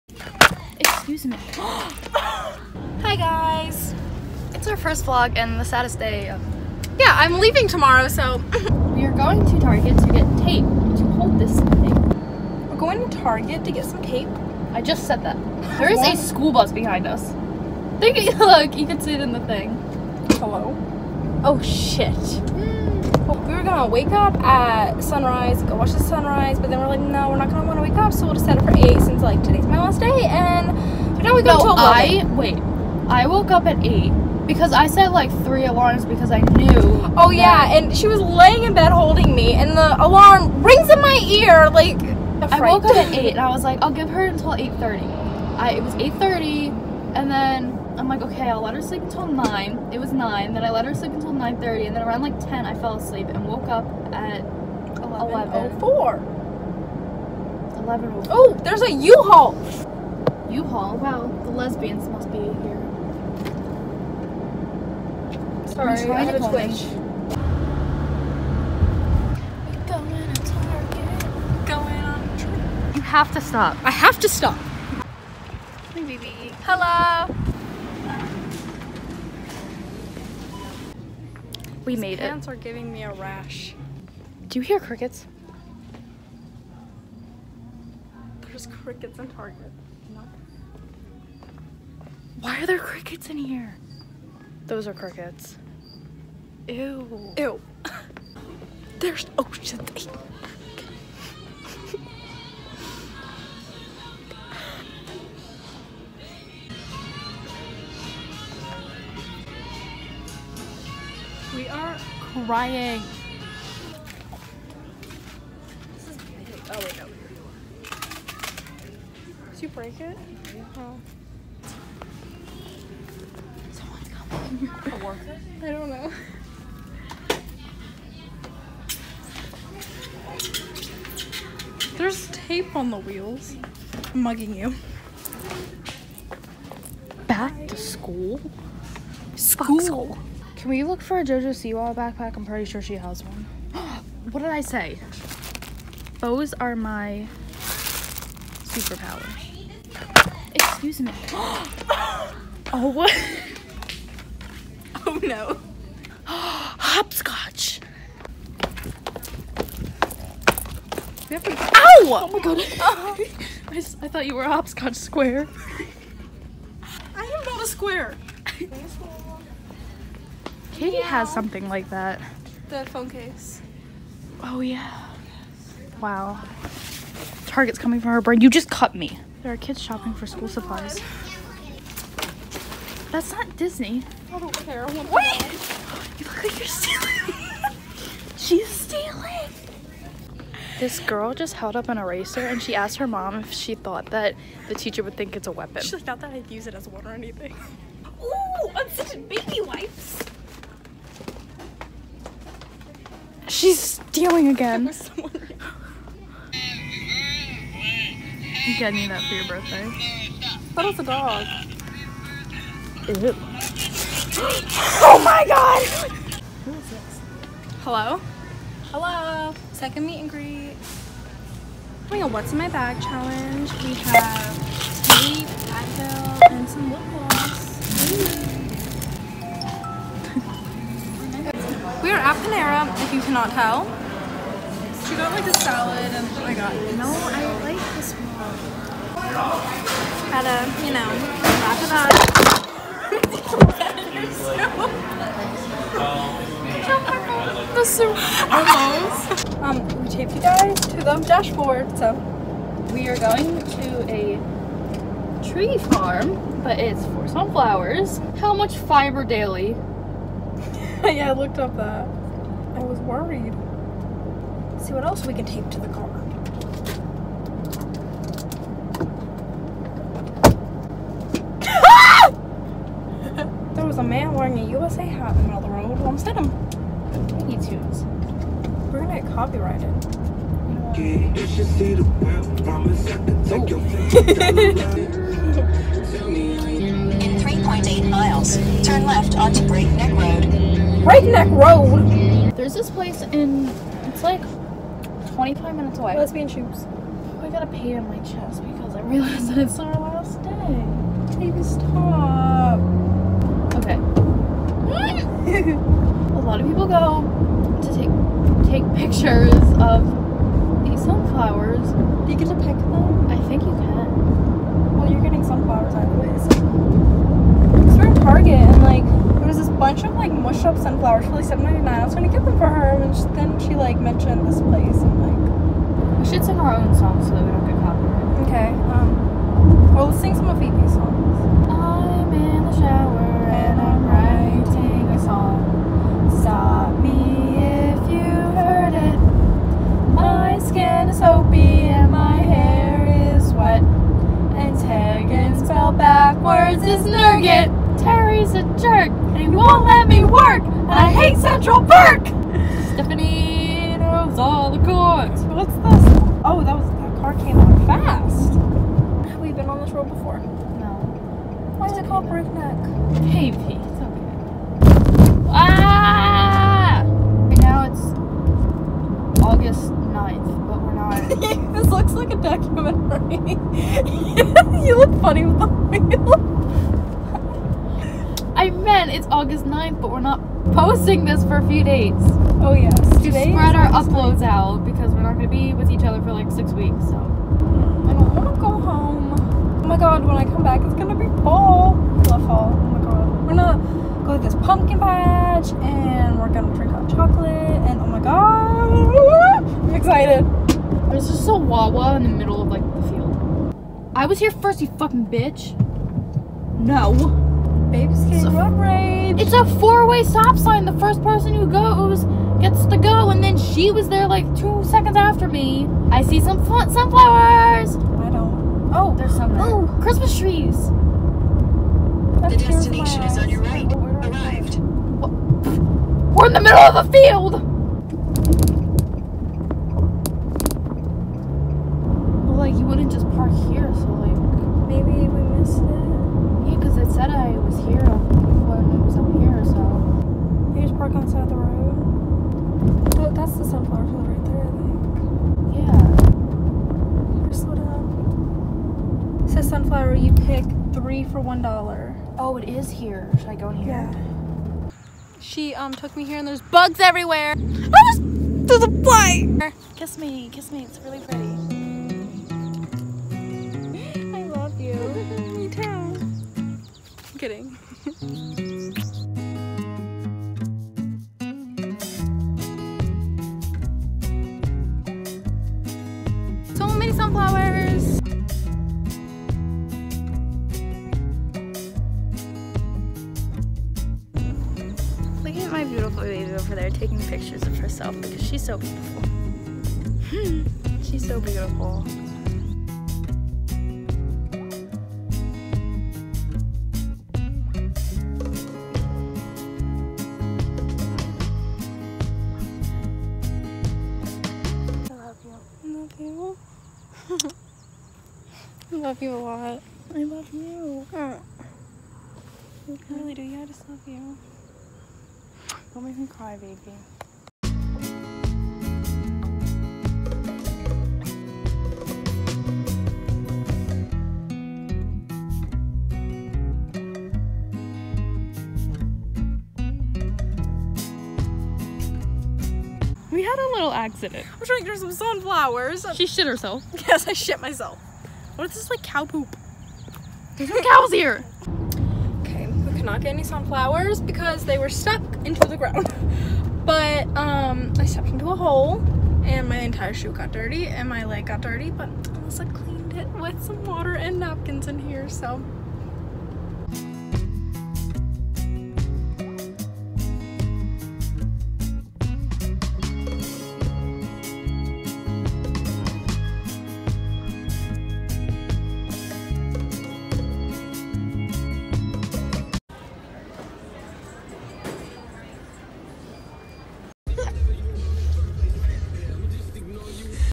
SUNFLOWER FIELD ON OUR LAST DAY // VLOG
Two weeks ago we had to say goodbye again. Since we won't be with each other for around 5 more weeks, we decided to post this vlog later than we filmed it so you don't have to wait longer for another upload. We're gonna continue our channel when we see each other again, but until then we hope you enjoy this video!

Cheyenne ~ @caiidreams
Alyssa ~ @alyssa.davis15